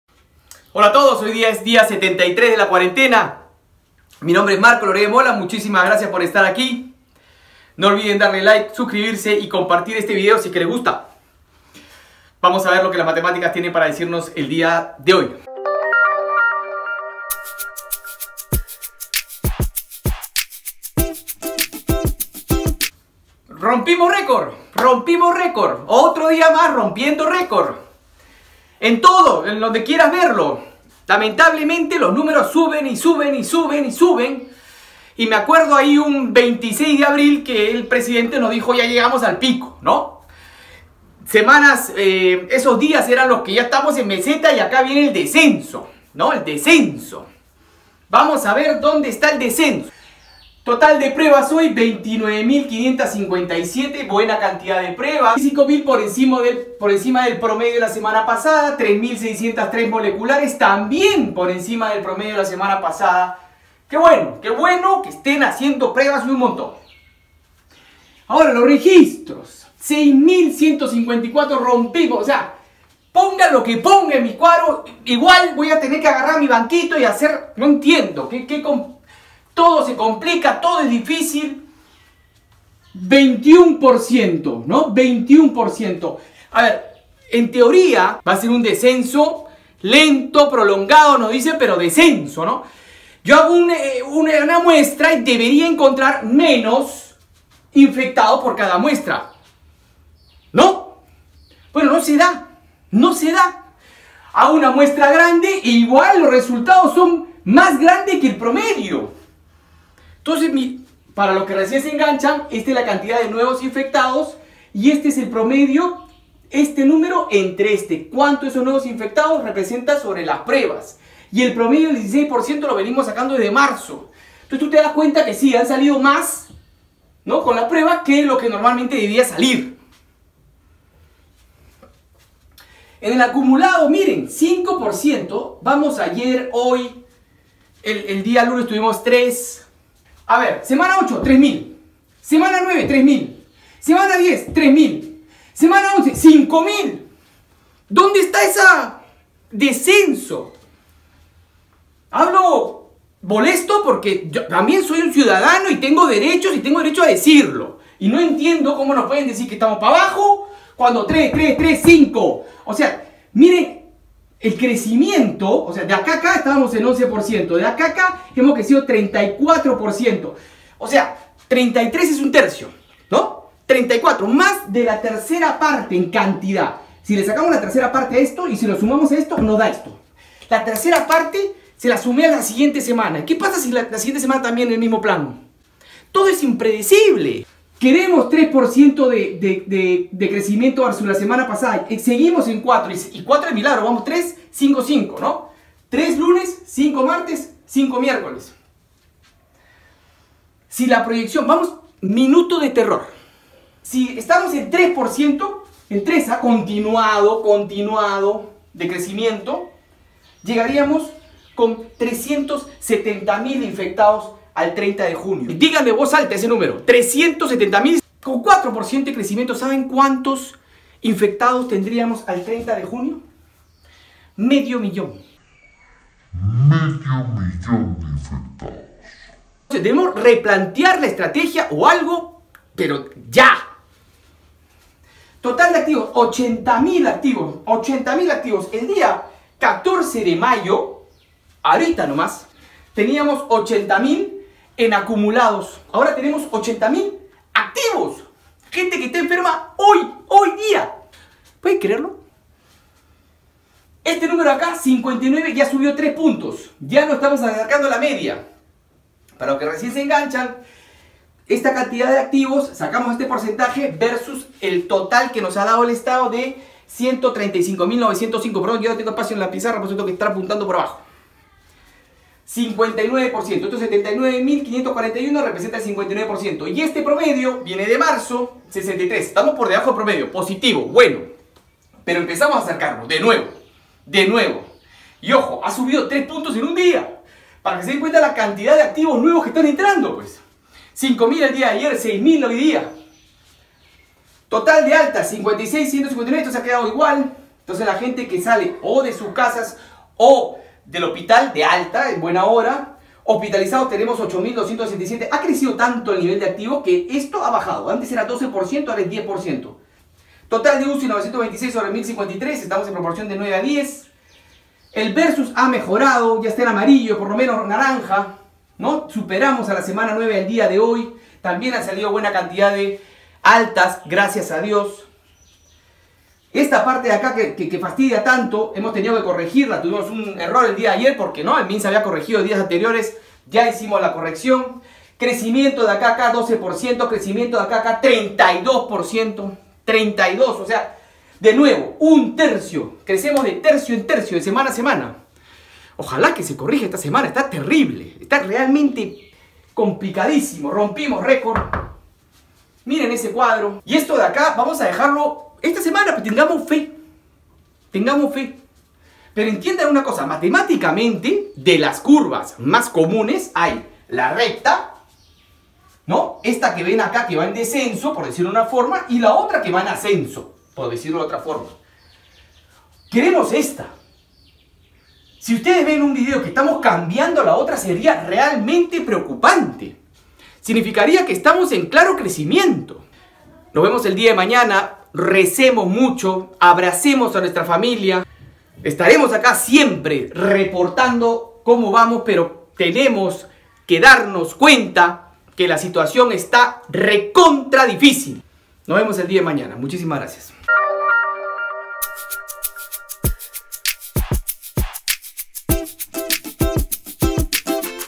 27 de Mayo 2020 - Perú ¿Cómo vamos?
27 de Mayo 2020 - Perú ¿Cómo vamos?

Análisis de Perú sobre la Pandemia
Marco Loret de Mola Director de MatLab
Necesitas clases para tus hijos?
Llama al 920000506 Primaria y Secundaria